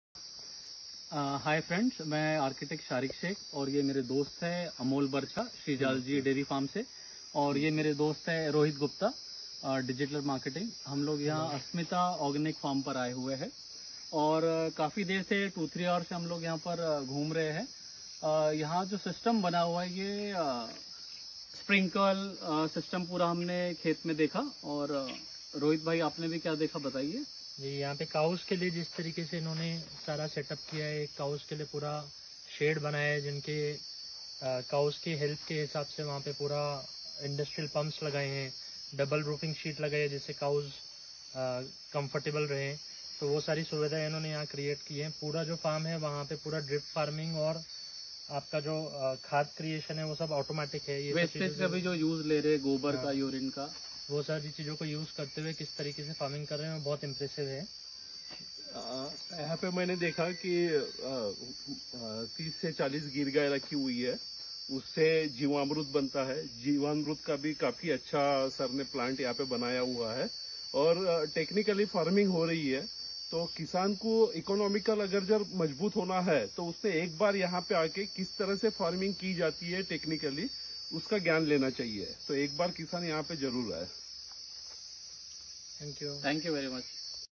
Ar. Shariq Sheikh, Shreelalji dairy farm Shri Amolji & Rohit Guptaji visited Asmita Organic Farms
Architect Shariq Sheikh, Shreelalji dairy farm Shri Amolji and Rohit Guptaji from Nagpur visited Asmita Organic Farms.